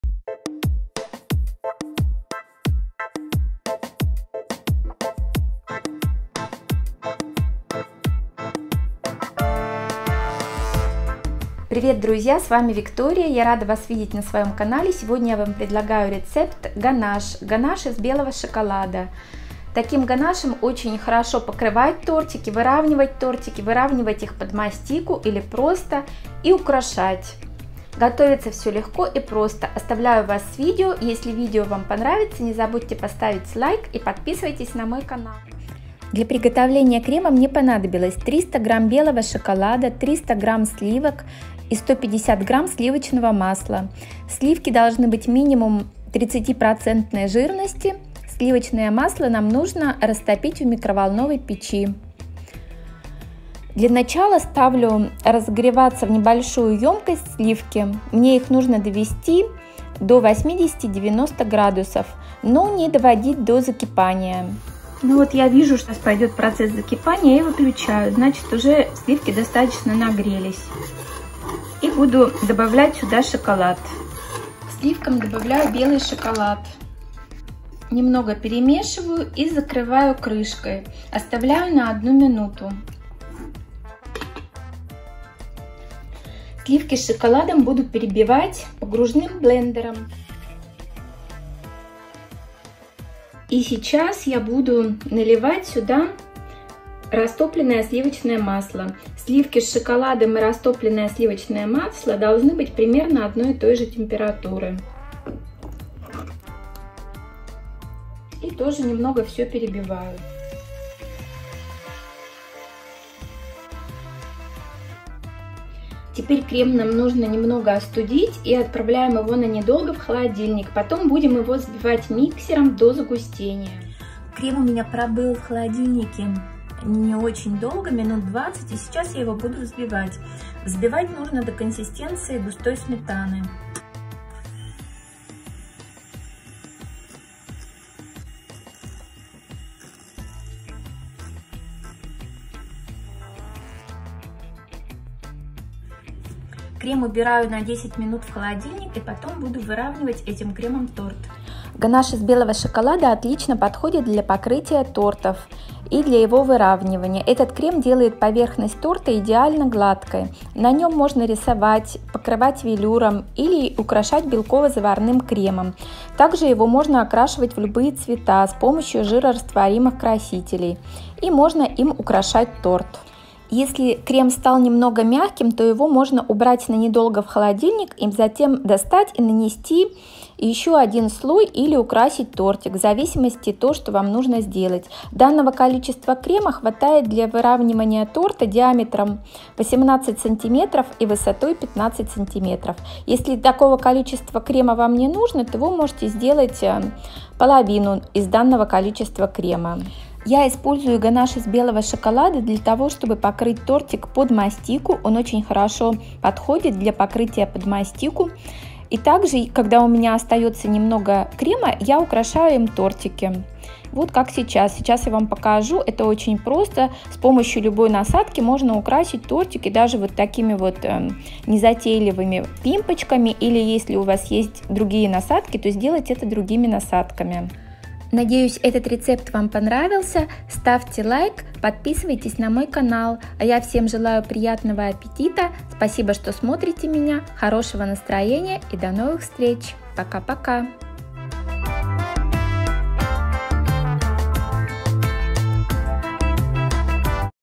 Ганаш! Шоколадный ганаш - Крем для Торта // Ганаш из Белого Шоколада
Друзья, Сегодня мы приготовим Шоколадный Ганаш - Ганаш из Белого Шоколада .
Этот Крем для Торта ,Идеально подходит для Выравнивания Тортов и Покрытия Торта под Мастику. А также им можно Украшать Торты , Кексы , Маффины и другие Вкусняшки !

Сборка рецептов домашней выпечки на любой вкус!!!

ГАНАШ 🧁🧁Шоколадный ганаш🧁🧁 Крем для Торта

Ингредиенты :

Шоколад белый - 300 г
Сливки - 300 г (минимум 30%жирности)